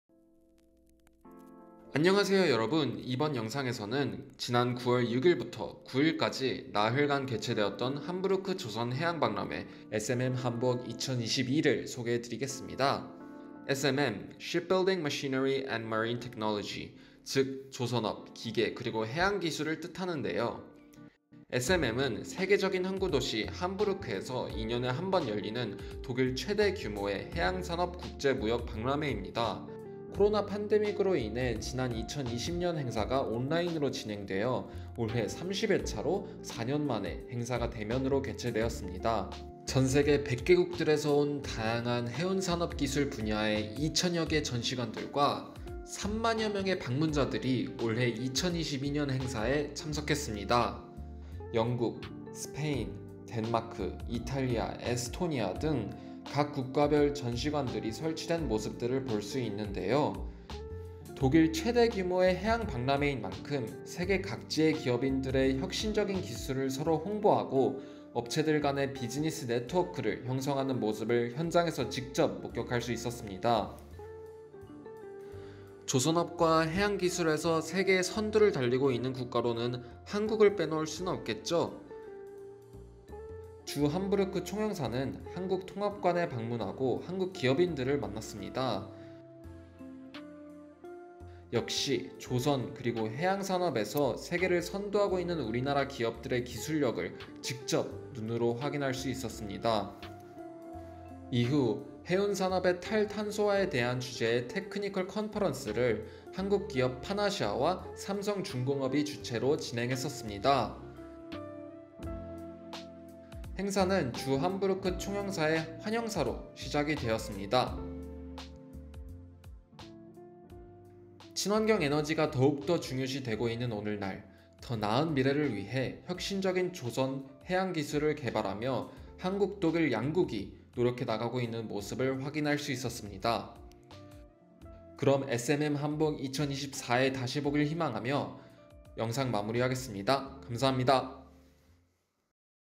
세계 3대 해양박람회, SMM Hamburg 2022를 소개합니다!
이번 영상에서는 지난 9월 6일부터 9일까지 나흘간 개최되었던 함부르크 조선 해양박람회 SMM Hamburg 2022를 소개해드리겠습니다!